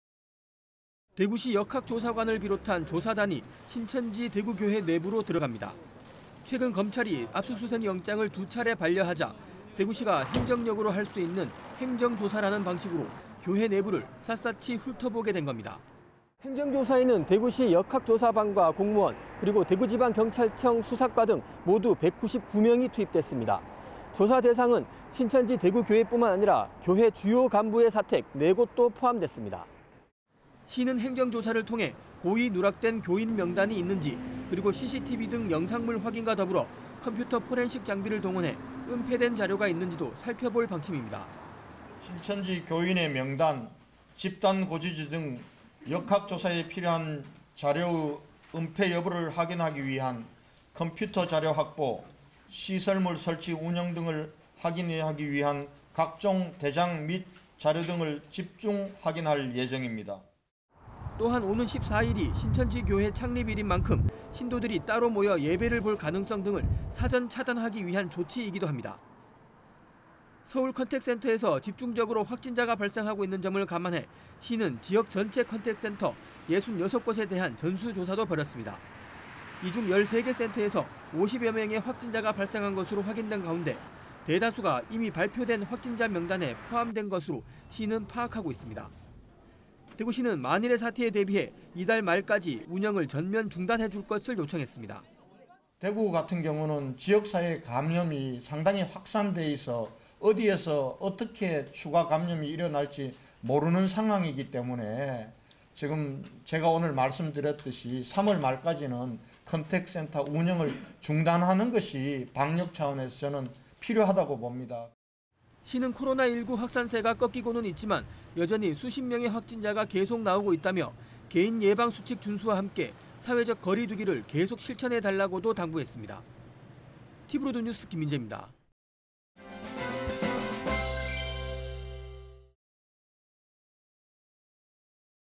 [자막뉴스] 신천지교회 행정조사 돌입… 은폐 여부 등 확인 / 티브로드
대구시 역학조사관을 비롯한 조사단이
신천지 대구교회 내부로 들어갑니다.

최근 검찰이 압수수색 영장을 두 차례 반려하자
대구시가 행정력으로 할 수 있는
행정조사라는 방식으로
교회 내부를 샅샅이 훑어보게 된 겁니다.

"행정조사에는 시 역학조사반과 공무원
그리고 대구지방경찰청 수사과 등
모두 199명이 투입됐습니다.
조사 대상은 신천지 대구교회 뿐만 아니라
교회 간부의 사택 4곳도 포함됐습니다."

시는 행정조사를 통해 고의 누락된 교인 명단이 있는지
그리고 CCTV 등 영상물 확인과 더불어
컴퓨터 포렌식 장비를 동원해
은폐된 자료가 있는지도 살펴볼 방침입니다.

INT) 권영진 / 대구시장
"신천지 교인의 명단, 집단 거주지 등
역학조사에 필요한 자료의 은폐 여부를 확인하기 위한
컴퓨터 자료 확보, 시설물 설치와 운영 등을
확인하기 위한 각종 대장 및 자료 등을
집중 확인할 예정입니다."

또한 오는 14일이 신천지교회 창립일인 만큼
신도들이 따로 모여 예배를 볼 가능성 등을
사전 차단하기 위한 조치이기도 합니다.

서울 컨택센터에서
집중적으로 확진자가 발생하고 있는 점을 감안해
시는 지역 전체 컨택센터 66곳에 대한 전수조사도 벌였습니다.

이중 13개 센터에서
50여 명의 확진자가 발생한 것으로 확인된 가운데
대다수가 이미 발표된 확진자 명단에
포함된 것으로 시는 파악하고 있습니다.

대구시는 만일의 사태에 대비해 이달 말까지
운영을 전면 중단해 줄 것을 요청했습니다.

"대구는 지역사회 감염이 상당히 확산되어 있어서
어디에서 어떻게 추가 감염이 이뤄질지
모르는 상황이기 때문에 3월 말까지는
컨택센터 운영을 중단하는 것이
방역차원에서 필요하다고 봅니다."

시는 코로나 19 확산세가 꺽이고는 있지만
여전히 수십명의 확진자가 계속 나오고 있다며
개인예방수칙 준수와 함께 사회적거리두기를
계속 실천해 달라고도 당부했습니다.

취재기자 : 김민재
영상취재 : 김송호